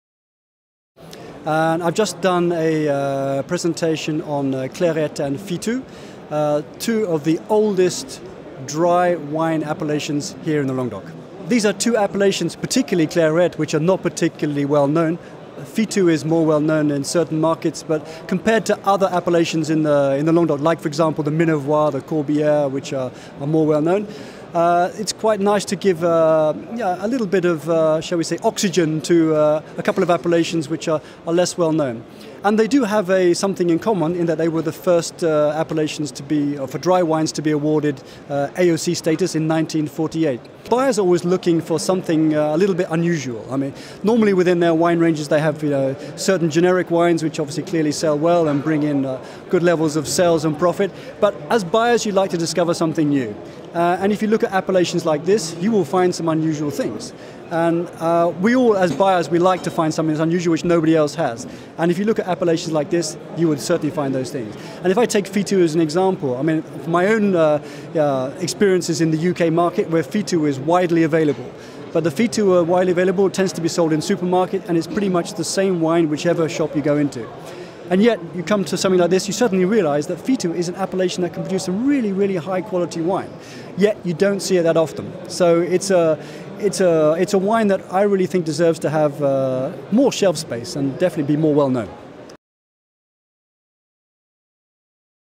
Fitou and Clairette du Languedoc : 65 years old and still going strong...!
« Fitou and Clairette du Languedoc : 65 years old and still going strong...! » presented by the CIVL - Languedoc Wines Joint Trade Council -- animated by Matthew Stubbs, Master of Wine